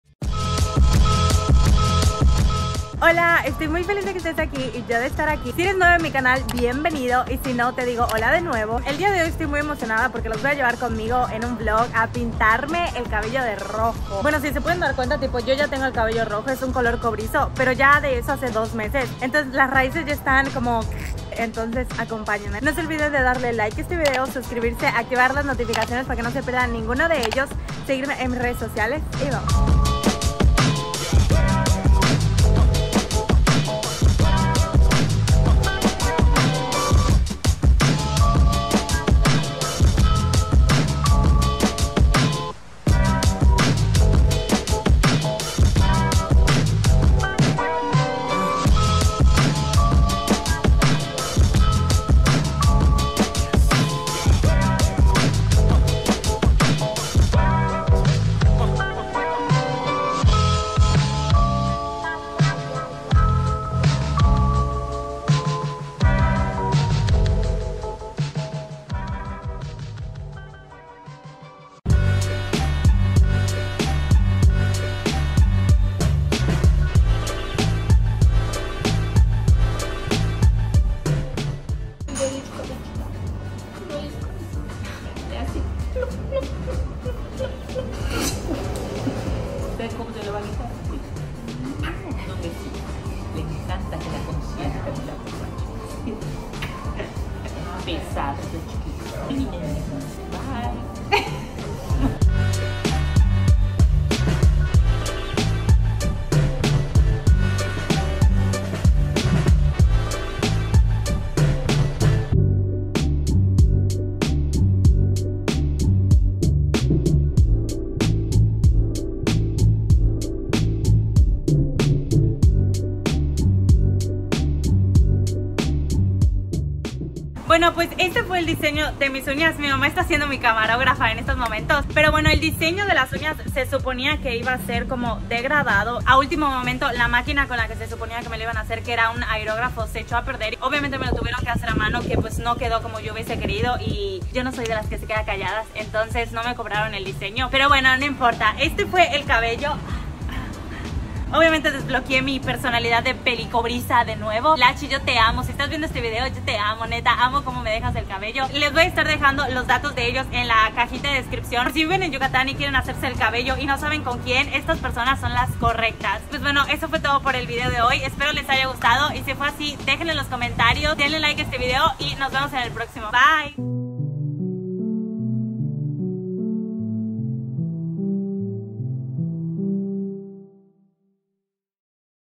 Acompáñame a pintarme el pelo de rojo | un día conmigo, cambio de look, cita de uñas
En el video de hoy los llevo conmigo a pintarme el cabello de rojo!, aunque se podría decir que fue un retoque porque ya me lo había pintado hace 2 meses, pero aún así super emocionada de desbloquear mi personalidad peli cobriza; al igual que los lleve a mi cita de uñas y me encantaría que me dijeran que les parece!!! Espero les guste mucho el video y díganme que otro tipo de videos quieren ver!

♡Edad: 21